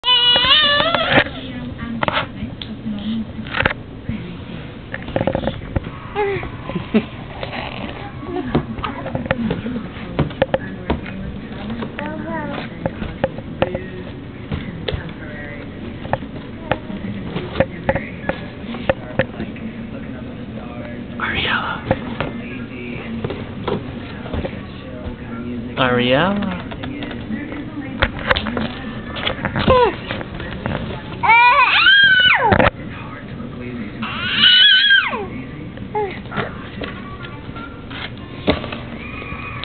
Talking to a box
Ariella Playing with her friends on the box